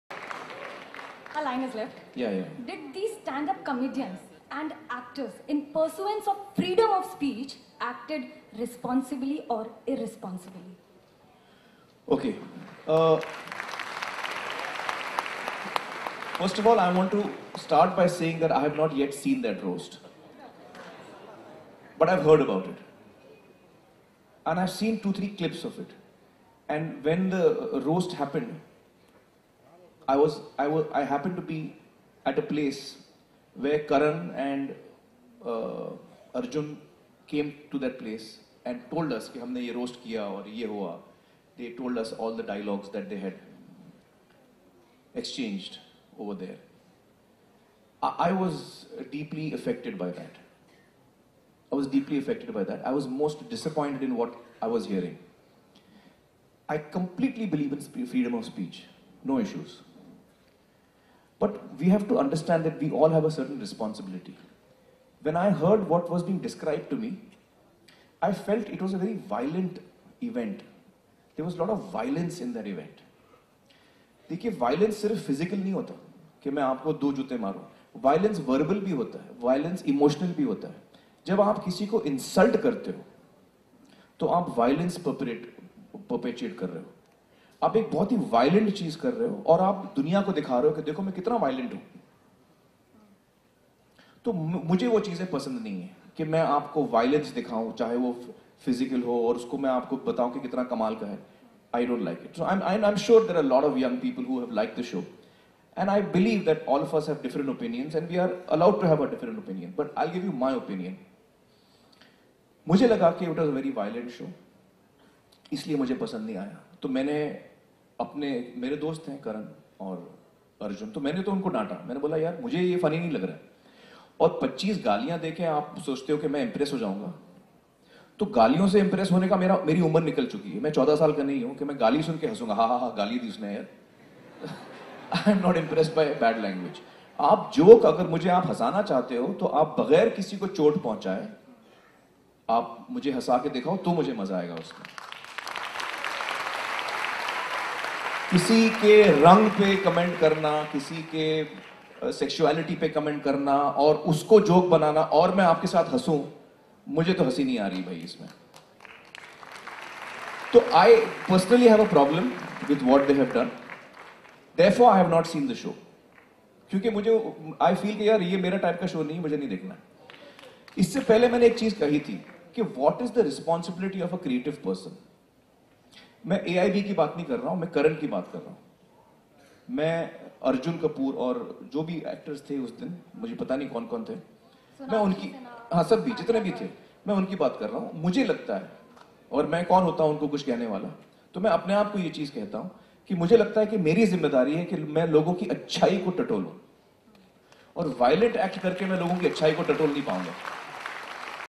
AIB KnockOut Controversy , Karan JOhar & Arjun Scolded By Me Says Aamir Khan
Always known to be a man who speaks his mind, and being assertive about every aspect of his life, Bollywood superstar Aamir Khan had a thing or two to say about the very controversial AIB Knockout Roast starring Karan Johar, Arjun Kapoor and Ranveer Singh, among others.
At yesterday's Youth for Governance 2015 against corruption event, he was asked by a member of the press about his take on the extremely popular, yet severely criticised show that was promoted on Youtube.